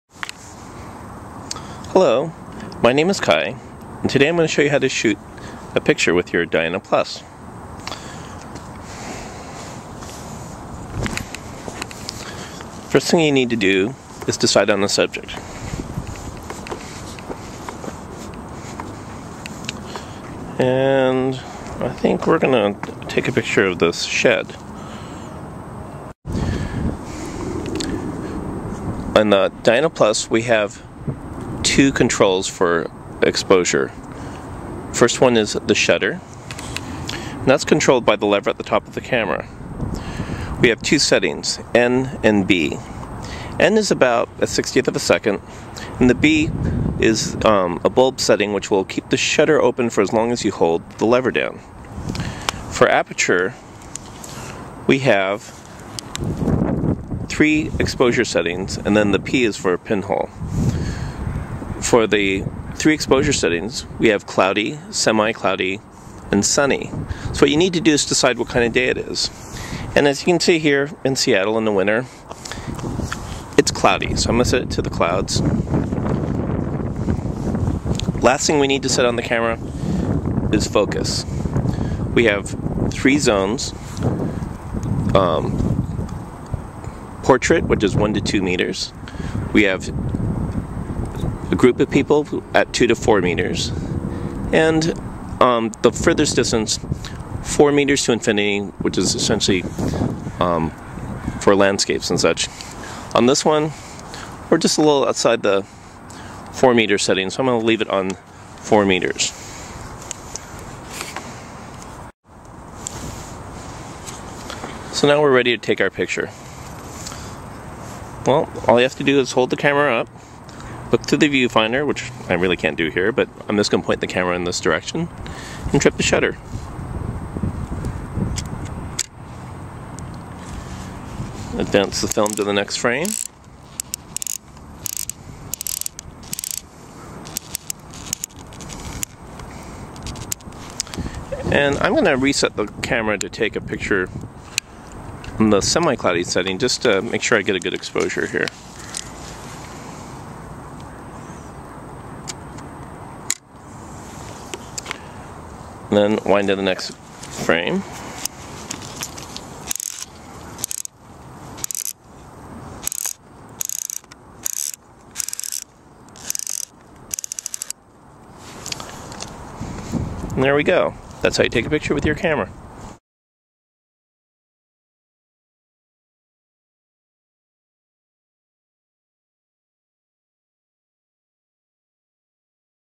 Shooting with Diana+
I show the basics steps to taking a photo with a Diana+. This will also work with vintage Diana, or any other basic plastic camera.